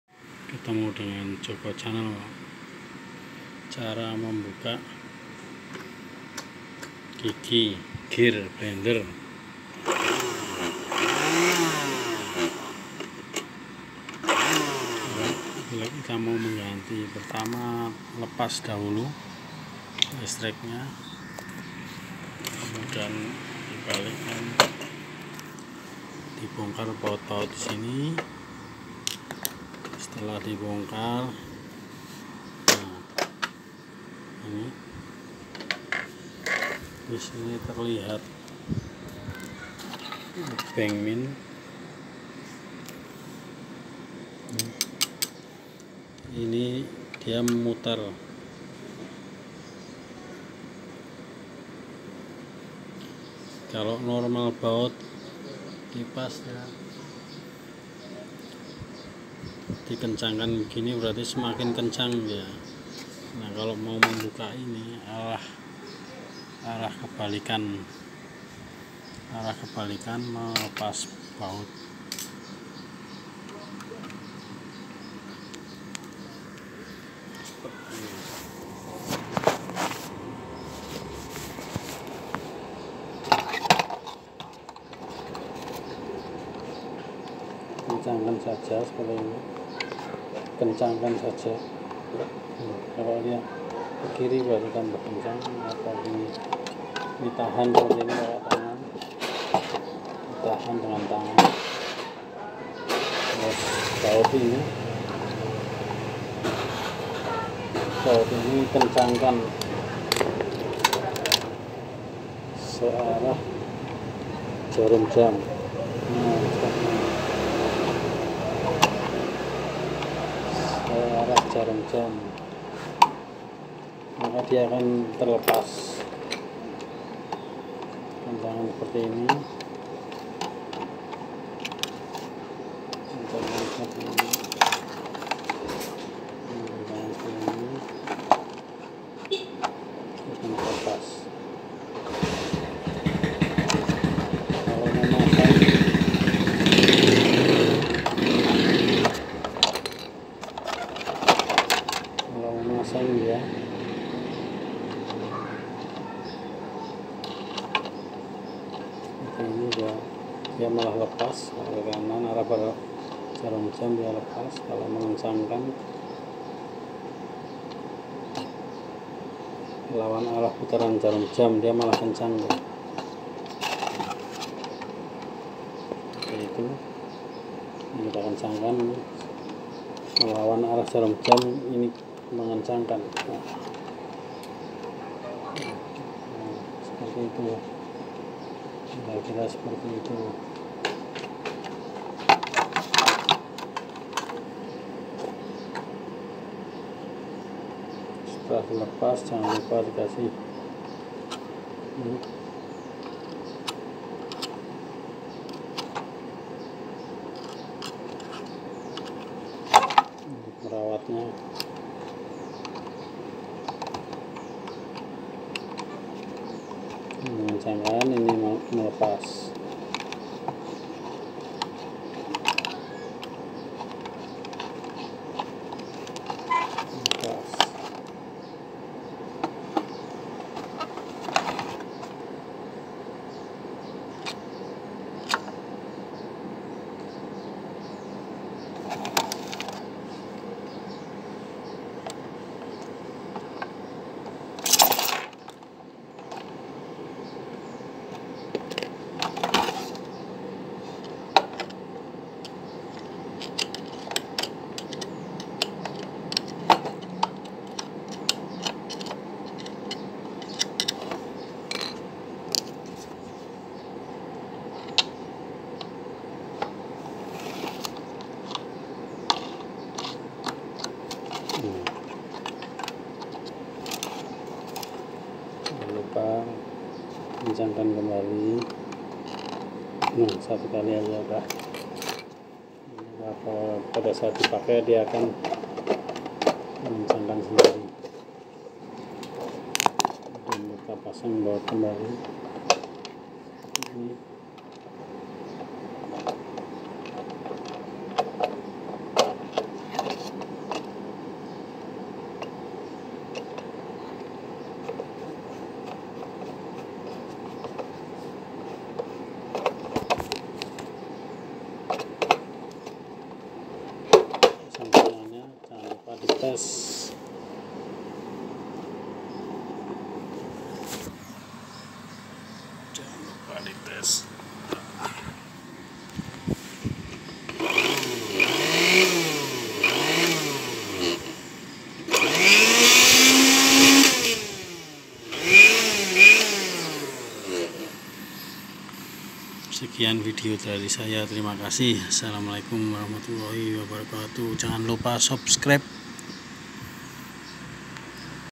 Cara melepas gear gigi pada blender.dan memberi pelumas
Pada saat melepas pastikan aliran listri sudah di lepas dan terbebas dari arus listrik.lalu buka cover belakang.setelah terbuka.putar dengan obeng minus searah jarum jam.dan gear blwnder tersebut ditahan dgn tangan.maka gigi gear alan terlepas.dan apabola akan.memasang putar ke arah berlawanan jarum jam.selamat mencoba.jangan lupa subsribe.🤲🙏🙏